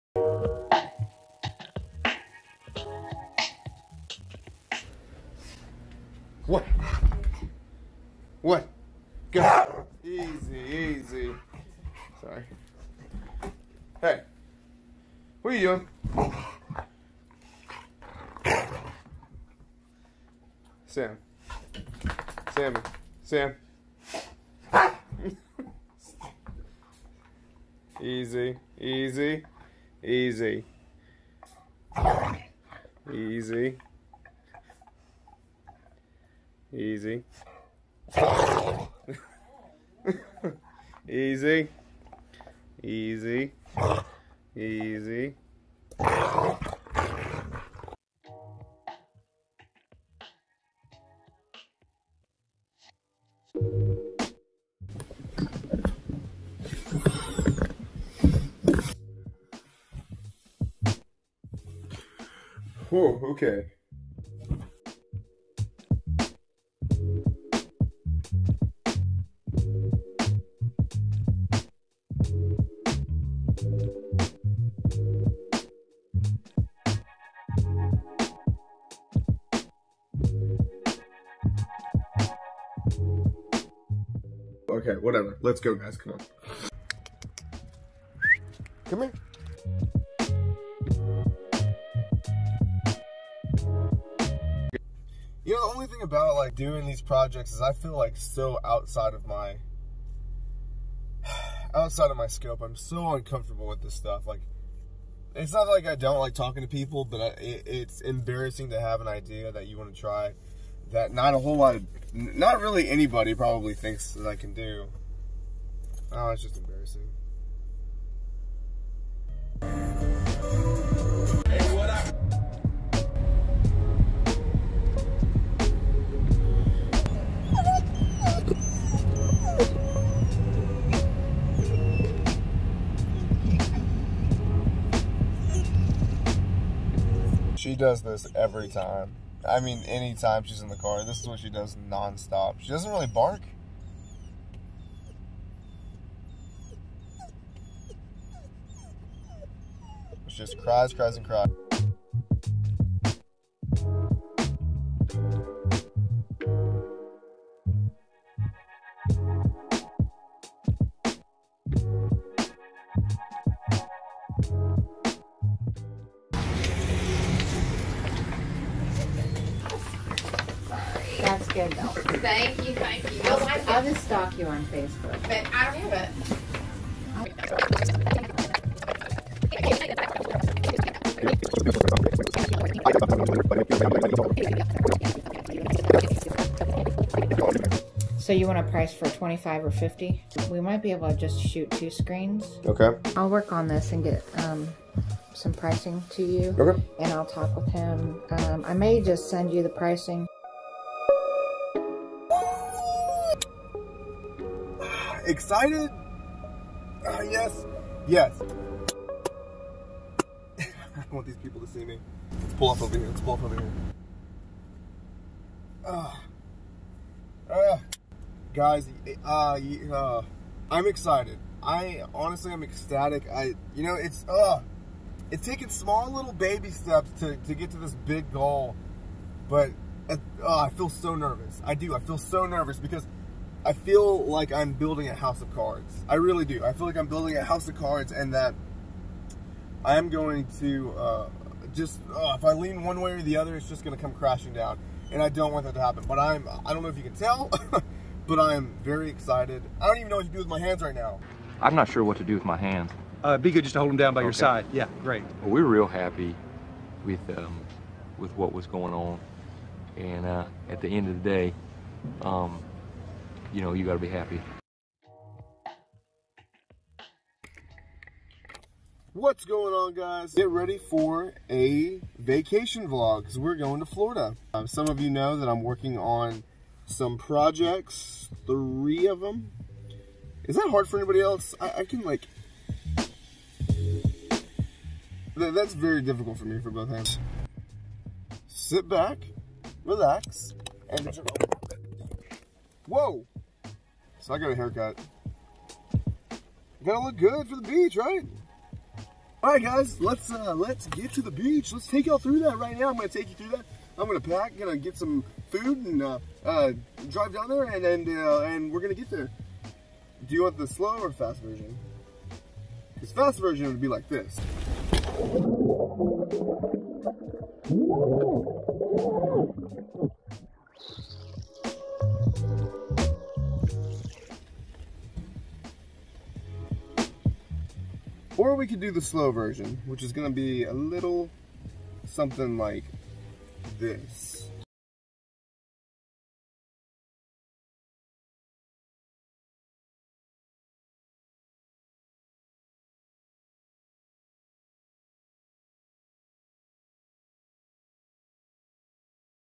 *NEW LOOK* | Making Plans For the Future | Traveling to Beach
Had some meetings and making headway on plans; trying new things; vlog number 15...finally starting to understand what being a Youtuber means